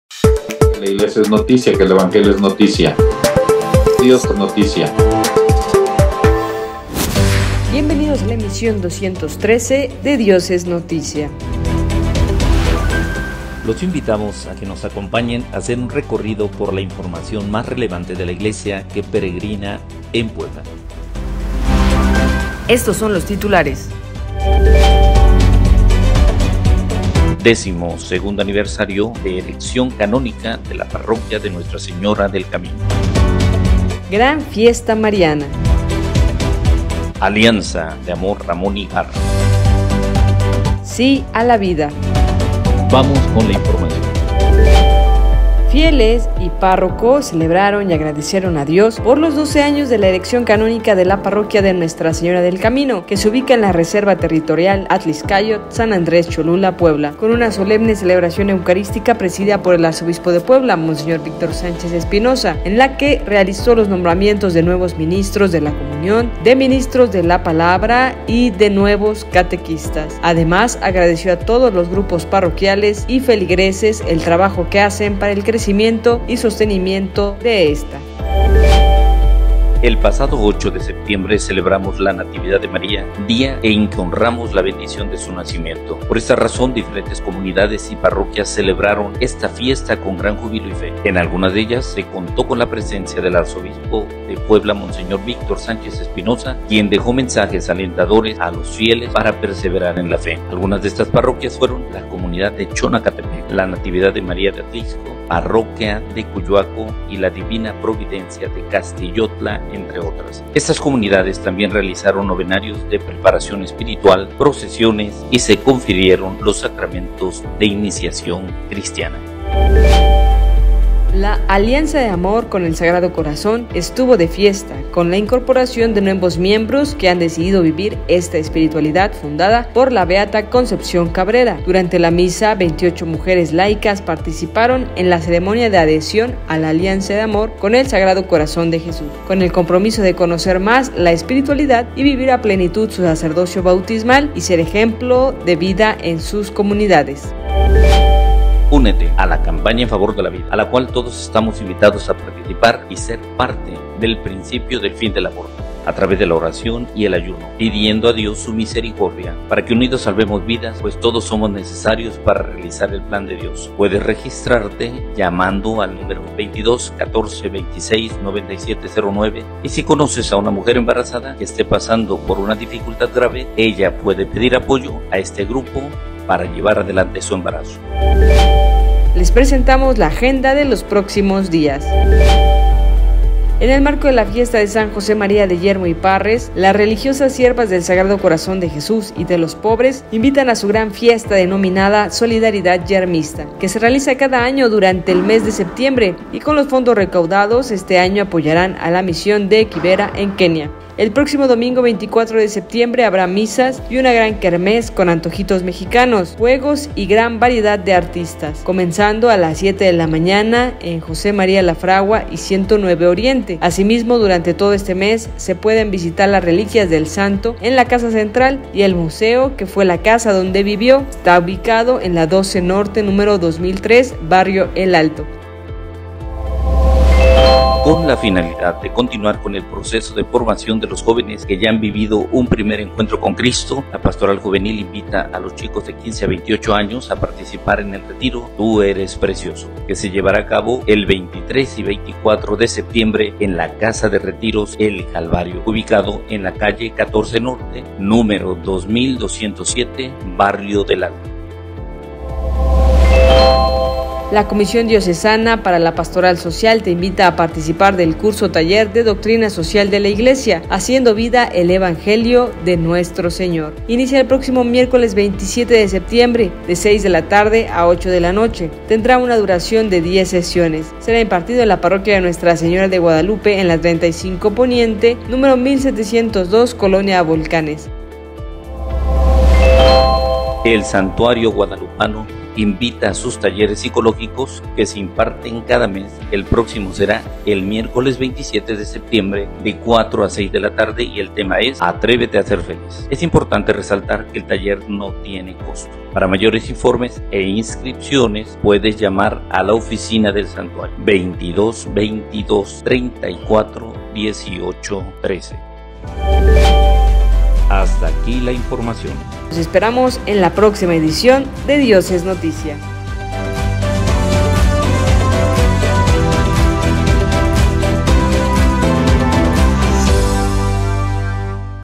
Dios es Noticia 213 - Arquidiócesis de Puebla
Noticias más relevantes de la Iglesia que peregrina en Puebla.

Descarga la app Arqui Puebla:
- Android: